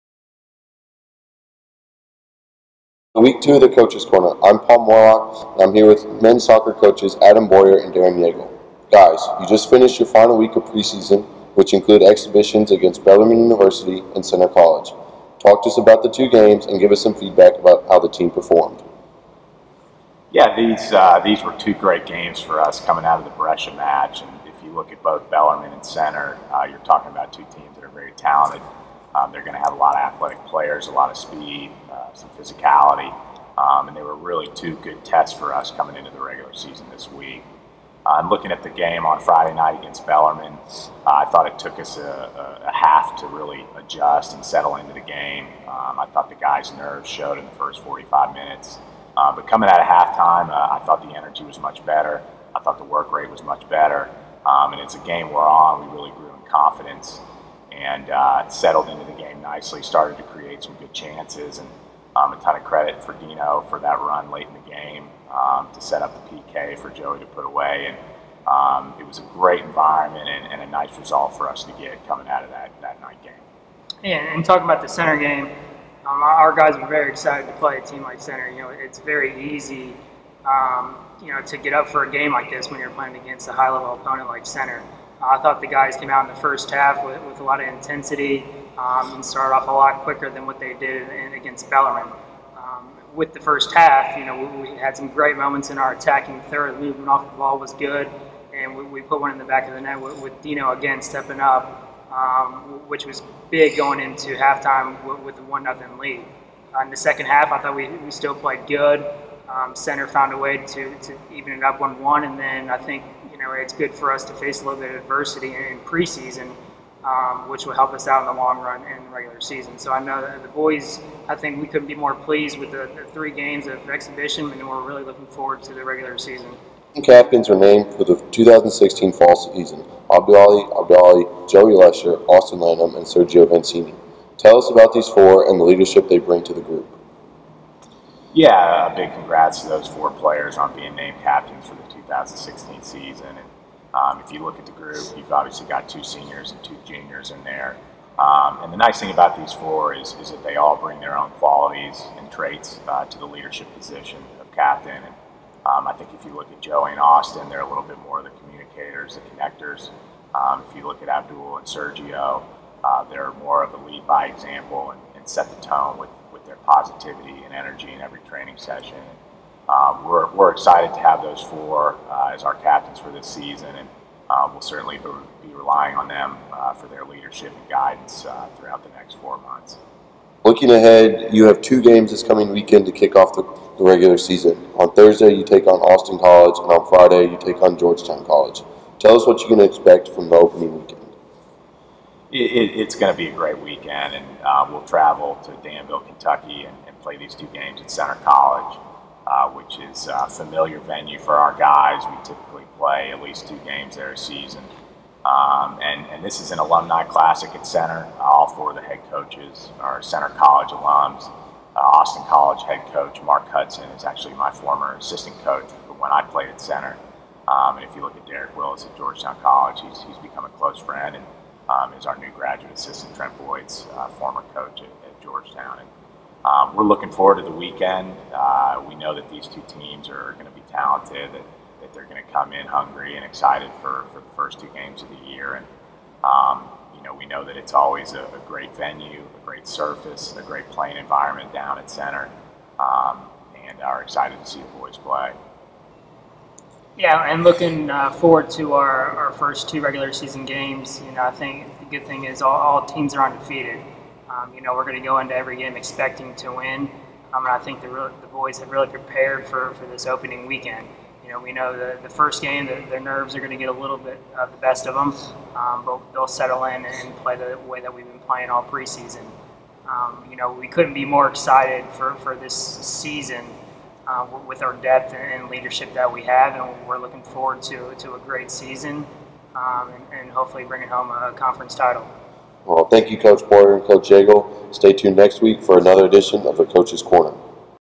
SUMS Coaches Corner -Week Two '16
Week 2 of the Coaches Corner with SU Men's Soccer coaches Adam Boyer & Darren Yeagle

Boyer & Yeagle review their past two exhibitions vs Centre & Bellarmine, discuss the four captains for the season, and look ahead to their opening regular season games against Austin College & Georgetown College.